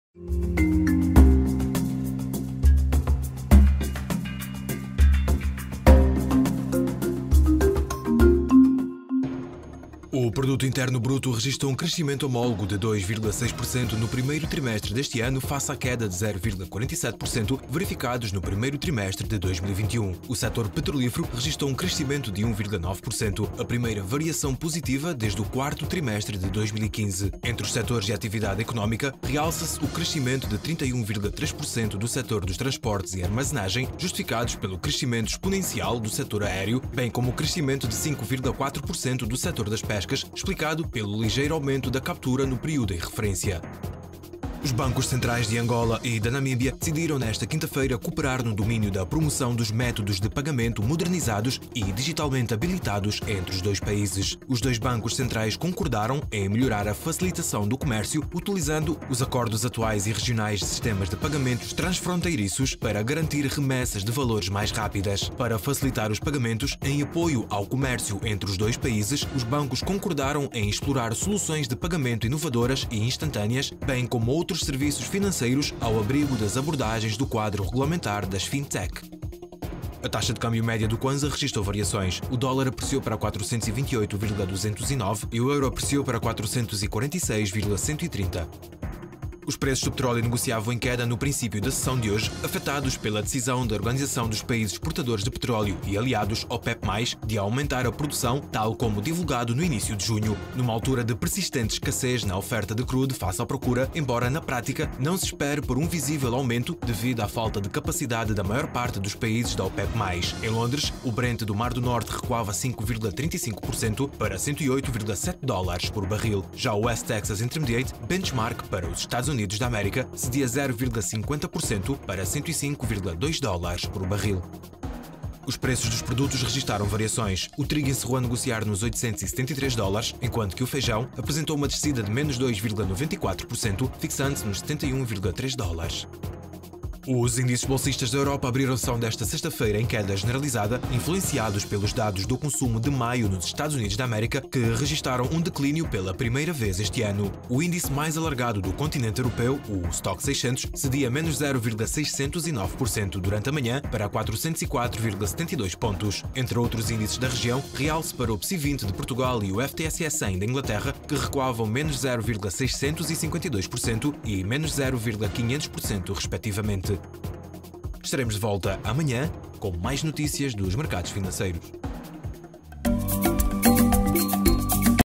Mercados ao Minuto BAI: 01 de Julho de 2022 (BAI)
Acompanhe no vídeo a edição do dia 01 de Julho do “Mercados ao Minuto BAI” e fique a par dos principais destaques do mercado financeiro e não só.